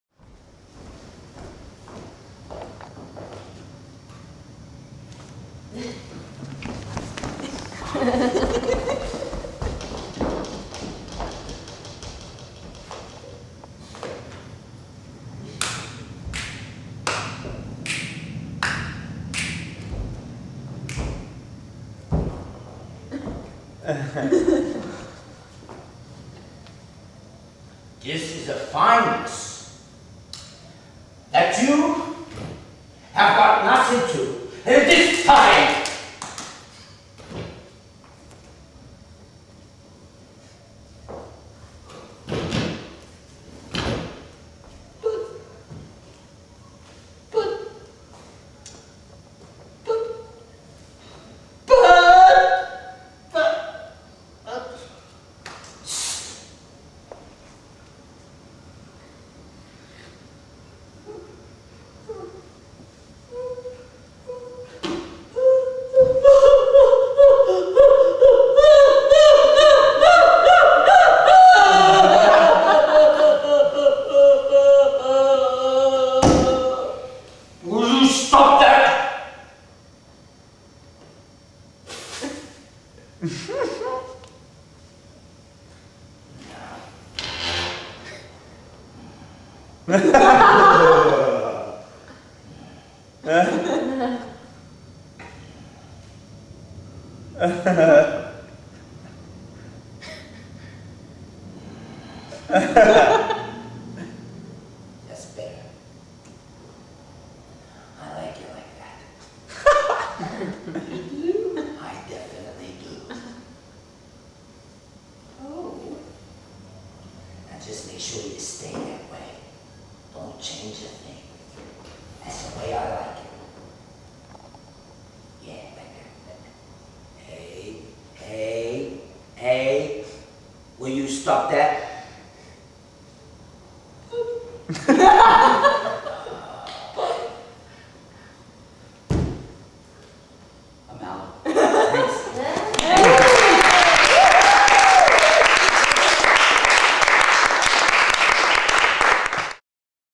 Harry and Ira
Ira Seidenstein and his friend and mentor Harry Heythorne, MBE.
Doing Ira's 'Laurel and Hardy' exercise in Ira's Acting Clown Workshop.
Cecil Street studios in Fitzroy, Melbourne, Australia.